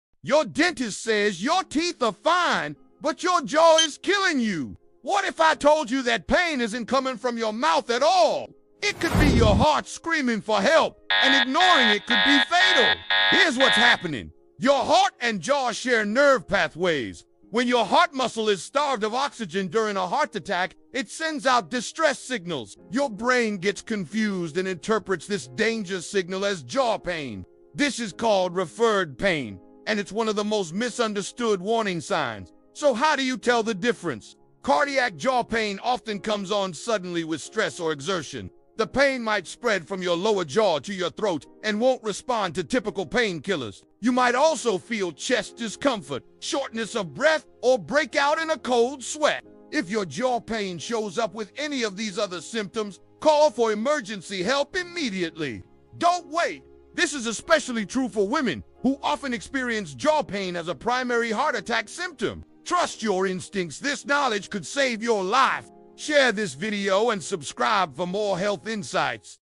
The Jaw Pain That Means Heart Attack (Not Dental Problems)
Your jaw pain might not be dental - it could be your heart sending an SOS signal.
Thousands of people dismiss jaw pain as a dental issue, never suspecting their heart is in danger. This critical medical knowledge could be the difference between life and death. Jaw pain is one of the most overlooked heart attack symptoms, especially in women, and understanding this connection is absolutely vital.
What You'll Discover:
✅ Why your heart and jaw share the same nerve pathways
✅ The exact characteristics of cardiac jaw pain vs dental pain
✅ How referred pain works in your nervous system
✅ Why women experience jaw pain more than chest pain during heart attacks
✅ When jaw pain requires immediate emergency response
Critical Warning Signs Explained:

Sudden jaw pain during physical activity or stress
Bilateral jaw discomfort spreading to throat and neck
Jaw pain that doesn't respond to typical pain relievers
Accompanying symptoms: chest pressure, sweating, nausea
Pain that comes and goes with exertion patterns

The Medical Reality:
Cardiac referred pain occurs because your heart and jaw nerves converge at the same points in your spinal cord. Your brain can't always distinguish where the pain originates, creating confusion that proves deadly. Studies show that up to 30% of heart attack patients, particularly women, experience jaw pain as a primary or sole symptom.
Time Stamps:
0:00 - The Jaw Pain Mystery
0:10 - The Heart-Jaw Nerve Connection
0:25 - How to Identify Cardiac Jaw Pain
0:45 - Life-Saving Action Protocol
Don't let misconceptions about heart attack symptoms cost you or a loved one their life. Classic chest-clutching heart attacks happen in movies - real heart attacks often present with subtle, confusing symptoms like jaw pain that most people ignore.